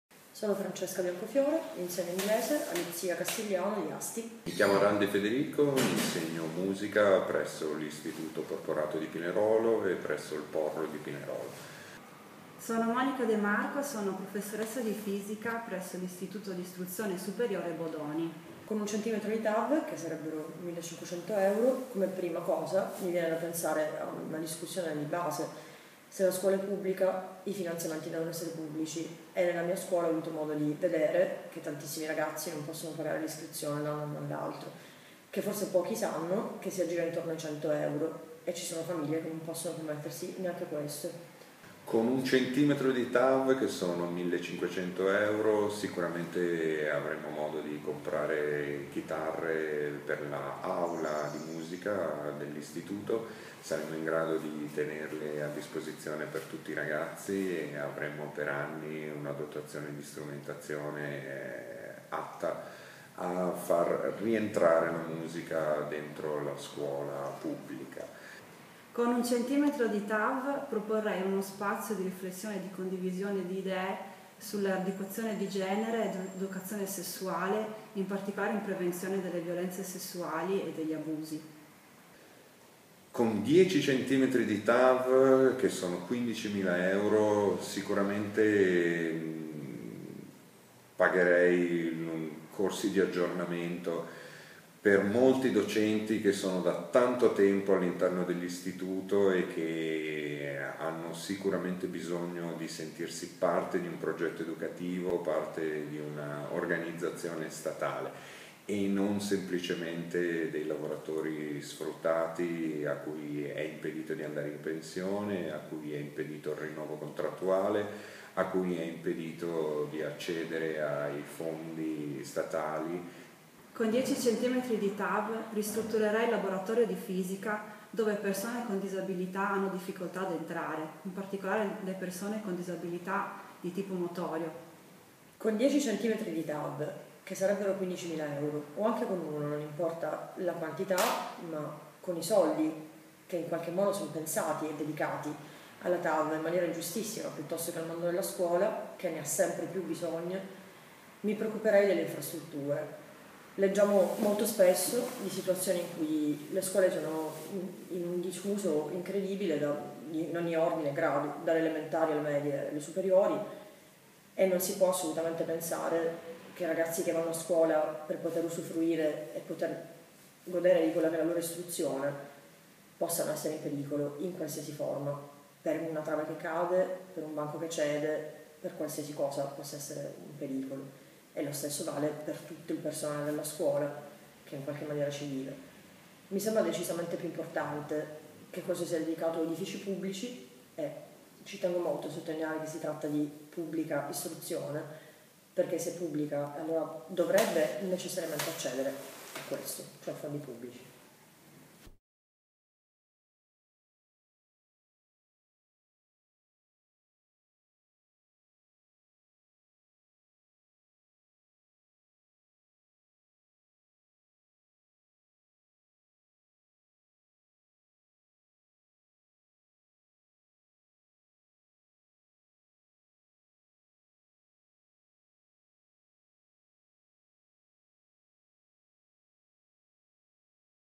Con1metrodiTav...salviamo la scuola pubblica!
Ogni euro speso per il Tav è un euro rubato alla scuola pubblica. Sullo sperpero del denaro pubblico nelle grandi opere.
con 1metrodiTav tu cosa faresti?
1 m di tav
158.712€